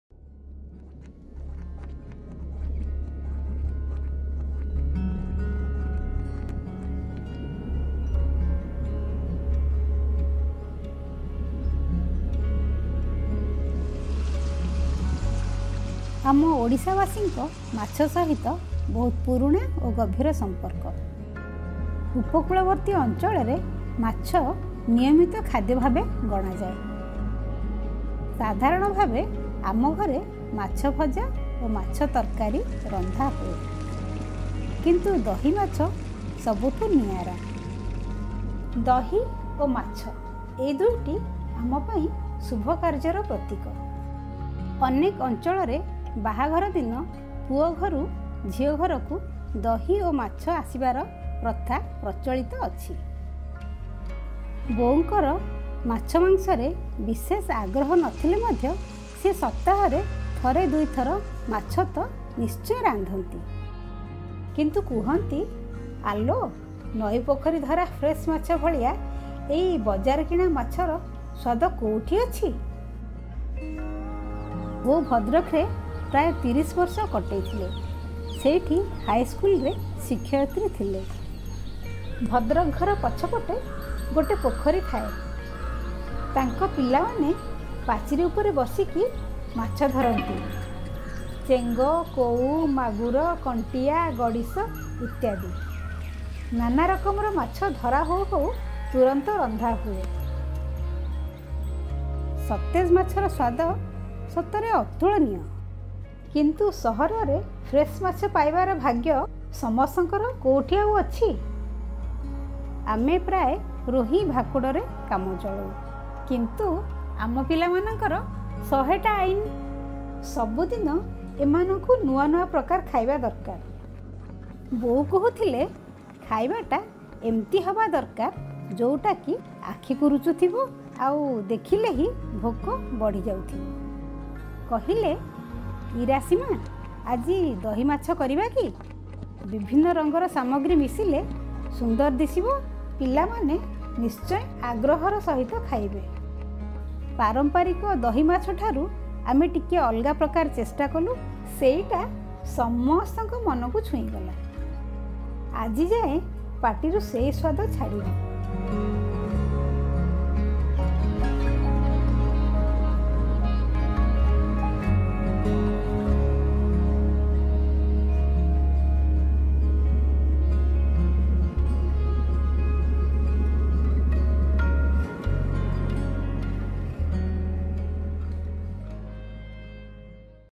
Dahi Machha - Episode 4 - Bou Kahuthile
Staple food for many Odias and a mainstay of wedding menus, fish is nothing short of a celebration for us. In fact, it is often part of the first meal that newlyweds have after the wedding rituals get over.

Since Odisha is blessed with a long coastline, we find various kinds of fish here. But our personal favourites are Rohu and Catla (besides the elusive Ilishi, of course). Among all fish preparations, Dahi Machha, or fish in a spiced curd gravy, holds a special place in our hearts. Typically, the Odia style of Dahi Machha uses distinctive ingredients such as mustard, garlic, and curry leaves. Our home-style recipe turns it into a visual feast with the addition of onion and tomato, thanks to our grandmother's penchant for experimentation!

Bou Kahuthile is a mini food series on our family favourites with a small serving of nostalgia. We will explore a few gems of Odia cuisine that our grandmother — Bou for the elders and Maa for us — loved to cook for the family. We lost her in April 1995, but her culinary legacy lives on.

Shot and edited: Raj Sampad
Script: Shilpi Sampad
Narration: Ira / Rajashree Mohapatra (Mommie)

Special thanks: Abodid (Creative guru), Mahesh Sahu (Odia typography and animation), Arnav Pati (Incendiary Films logo)